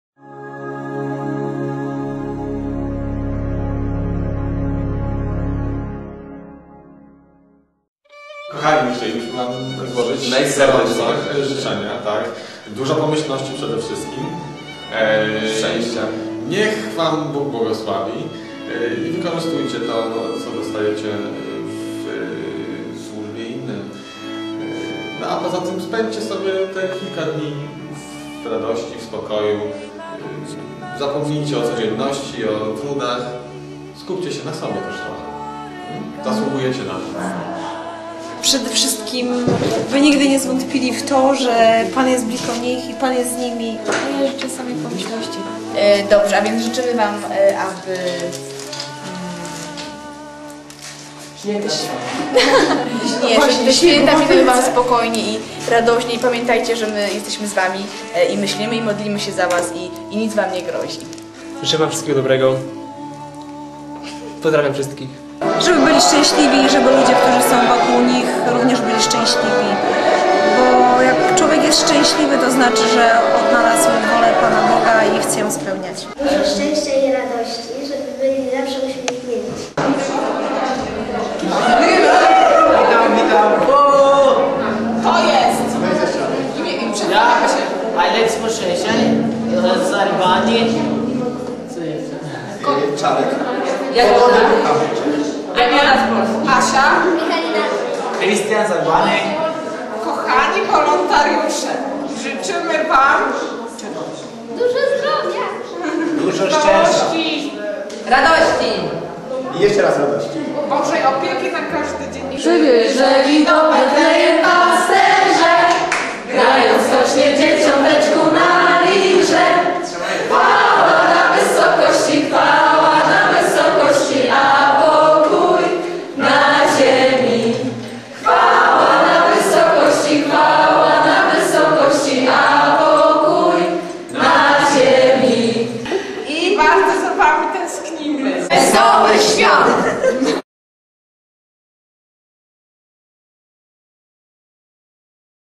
Żczenia dla misjonarzy
życzenia skladaja wolontariusze międzynarodowego wolontariatu don bosco